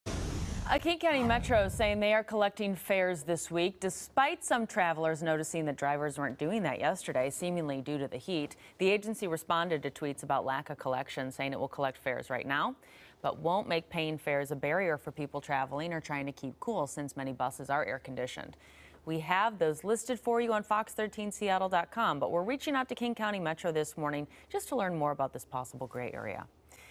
King County Metro address fare collection during heat wave | FOX 13 Seattle
The county said it won't collect fares on transit during the heatwave, which will allow people to get into cooling centers and access to other necessities.